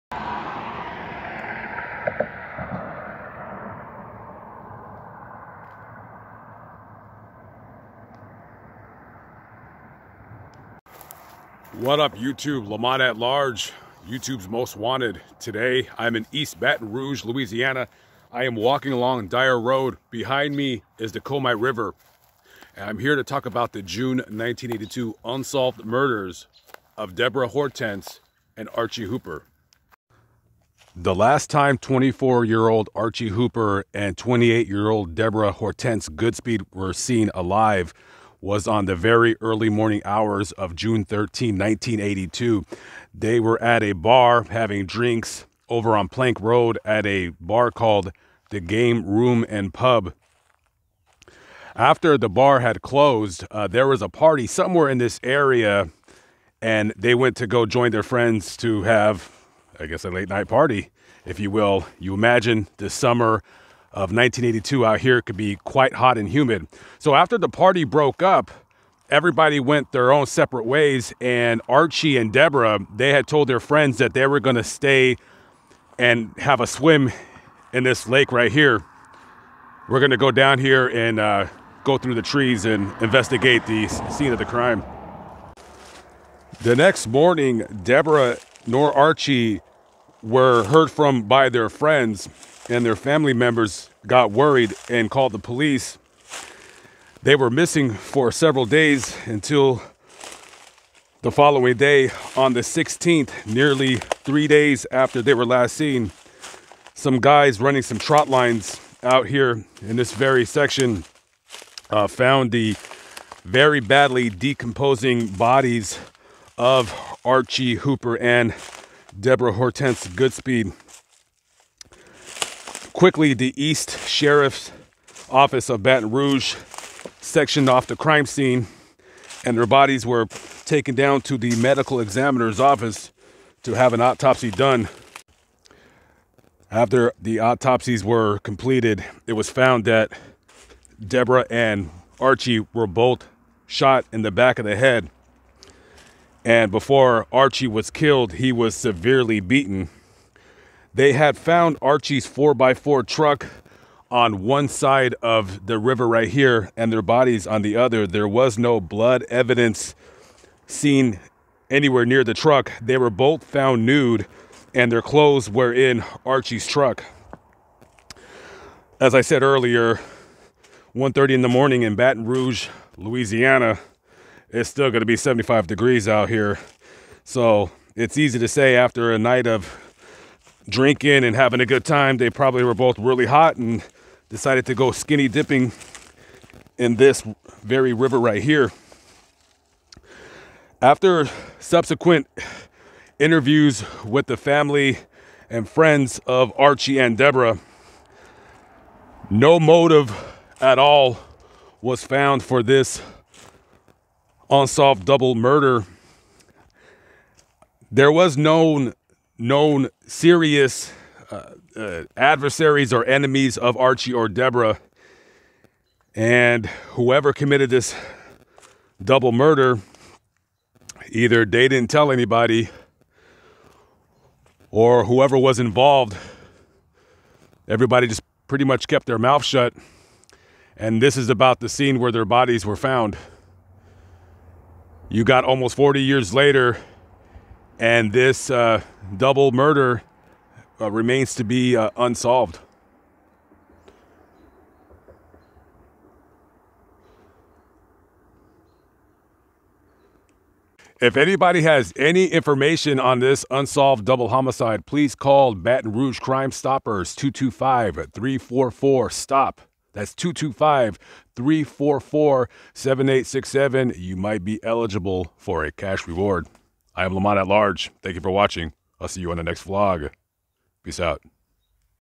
The Unsolved Murders of Archie Hooper And Debrah Hortence Goodspeed
talking about and visiting the location of the all most 40 year old unsolved double homicide of Archie Hooper and Deborah Hortense Goodspeed

if anyone has any info on this case please call crime stoppers baton rouge 225